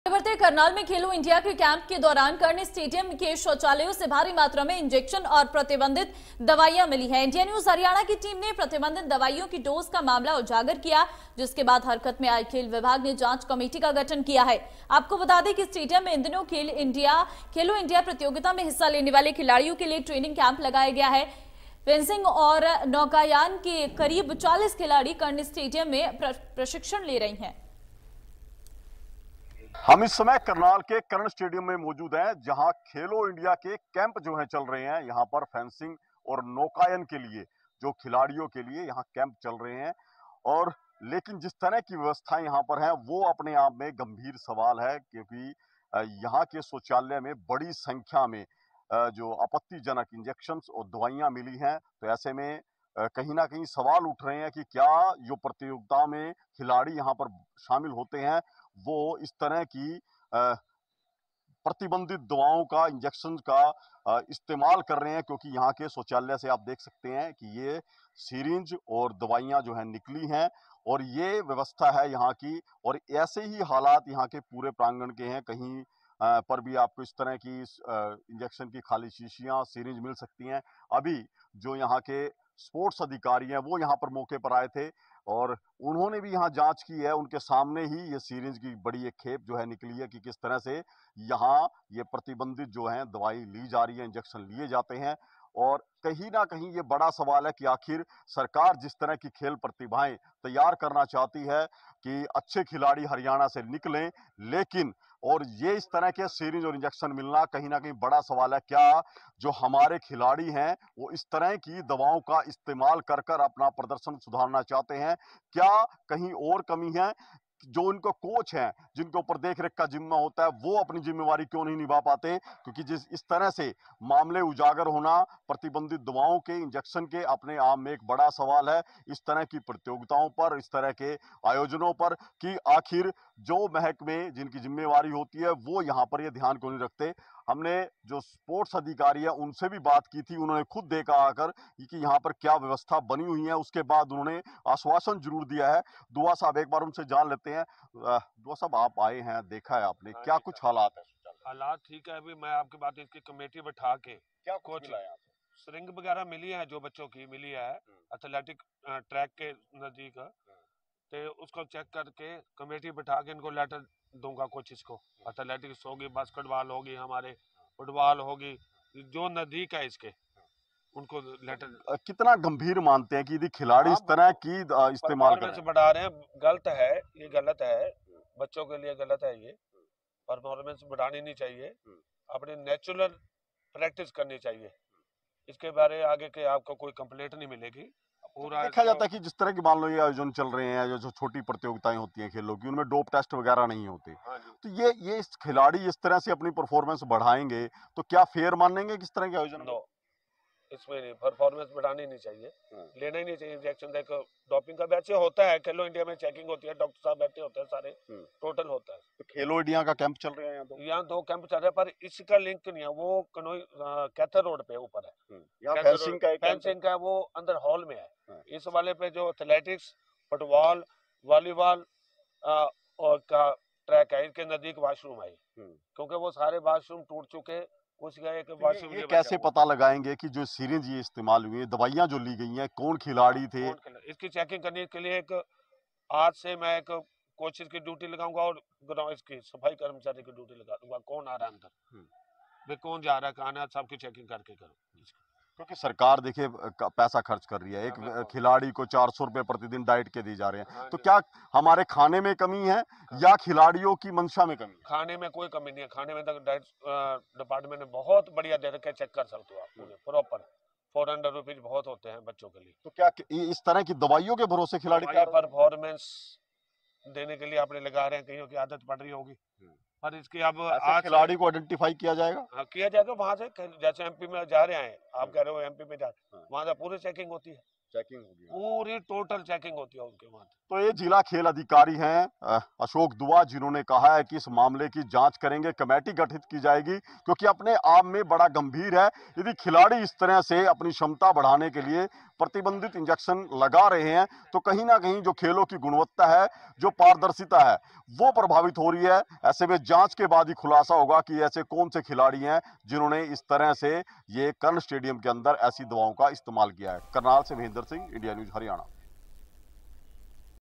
Haryana News : प्रतिबंधित दवाइयां और इंजेक्शन का मामला,खेल विभाग ने जांच के लिए कमेटी का किया गठन
Haryana News : प्रतिबंधित दवाइयां और इंजेक्शन का मामला,खेल विभाग ने जांच के लिए कमेटी का किया गठन | Hindi News